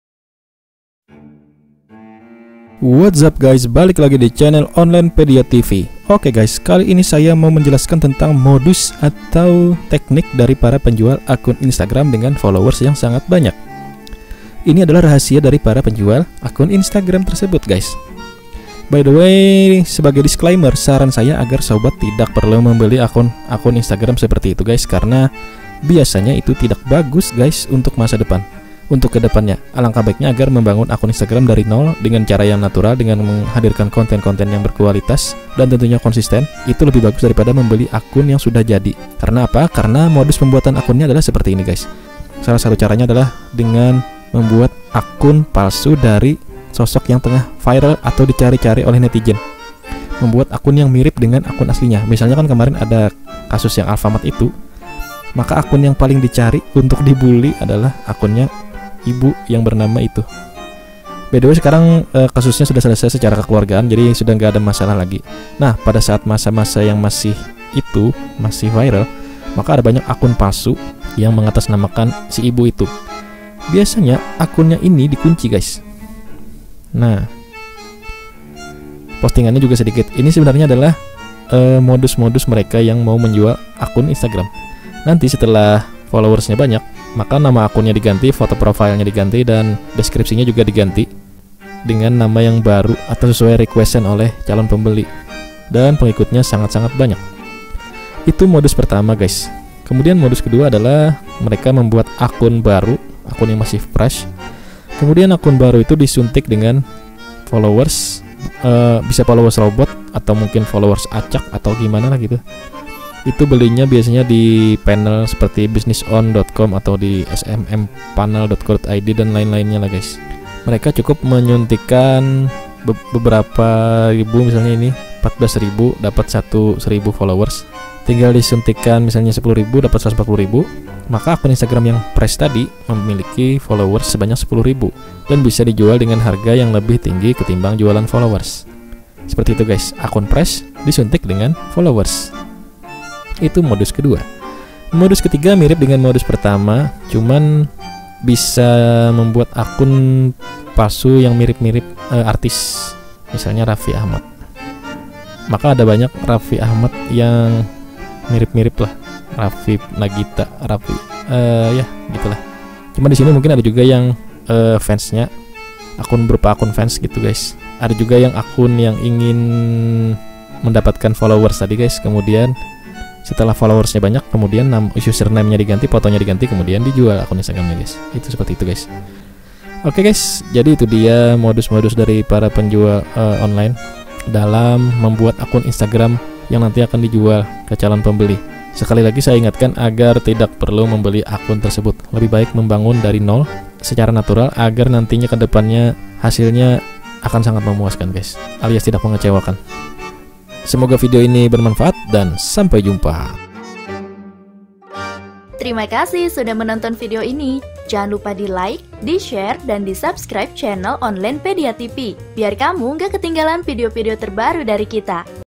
MODUS & TEKNIK PENJUAL AKUN INSTAGRAM BERFOLLOWERS BANYAK !! JANGAN DIBELI !!
Berikut ini teknik yang dilakukan oleh para penjual akun IG yang followersnya banyak. Sebaiknya jangan dibeli, yaa..

SUPPORT saya dalam membuat konten dengan :
- TAP tulisan "Terima Kasih" atau "Super Thanks" di atas kolom komentar

Silahkan berkomentar!